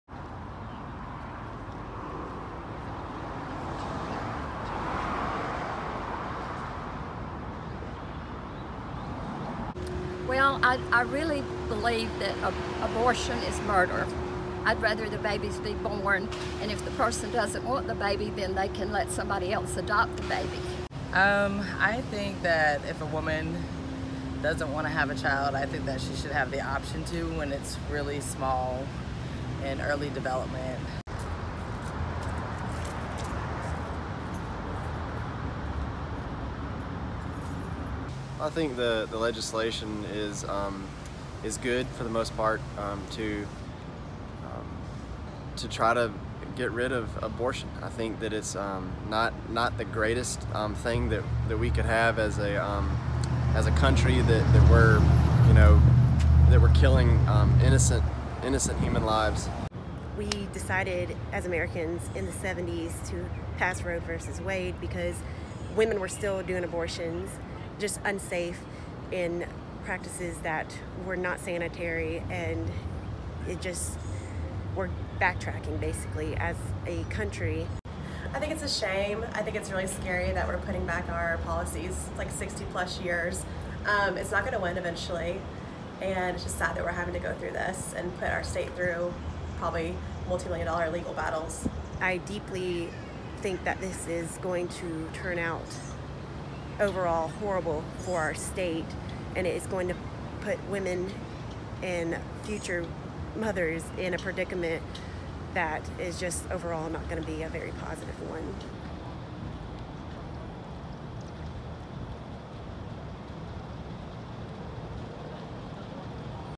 Ala. abortion ban law evokes sharp reactions
(16 May 2019) A day after Alabama Gov. Kay Ivey signed the state's most restrictive abortion bill into law, residents of the state are sharply divided over the issue.
"I really believe that abortion is murder. I'd rather be that the baby is born and if the person doesn't want the baby, then they can let somebody else adopt the baby, said Janis Lowther.
But for Devin Ammerman, a nursing graduate, the law will set the country backwards.
"I deeply think that this is going to turn out overall horrible for our state and it is going to put women and future mothers in a predicament that is just overall not going to be a very positive one," she said.
The Alabama abortion ban is the most far-reaching measures as some conservative states push new abortion restrictions in the hopes of getting a case before the conservative Supreme Court majority.
The abortion ban would go into effect in six months if it isn't blocked by legal challenges.